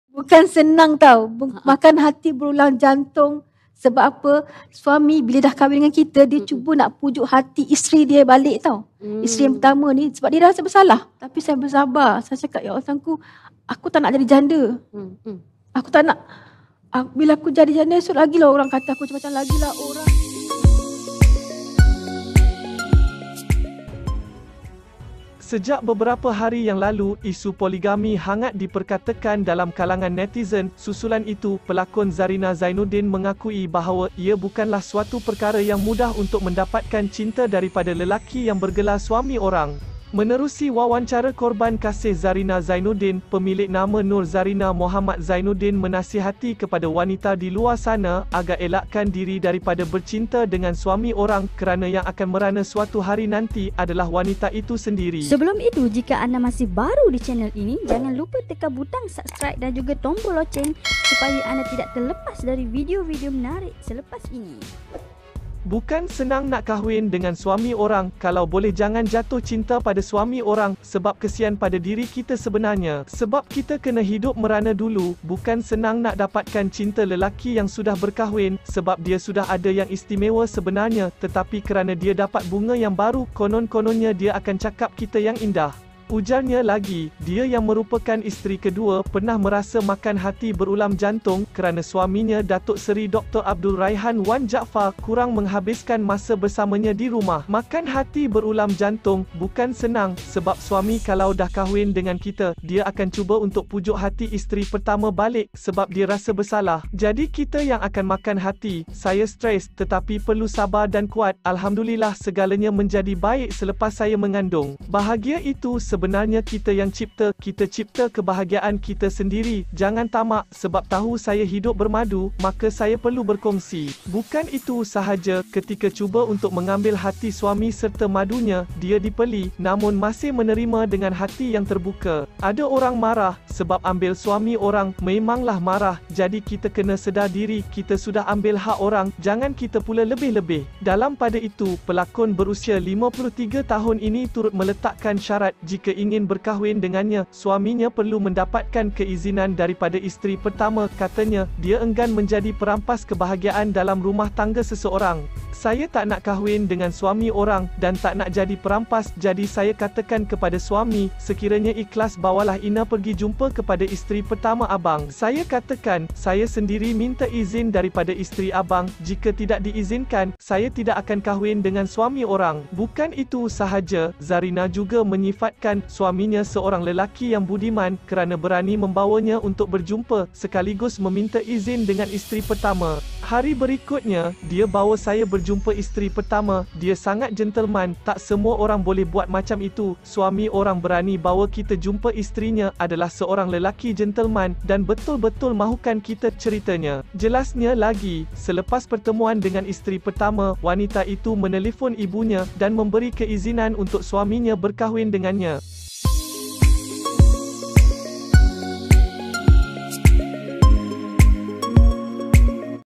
Bergelar ISTERI KE-2, Zarina Zainuddin PERNAH MERANA & DEDAHKAN INI... "Kita MAKAN HATI,Stress..."
📵 Editing : Samsung Note 20 Ultra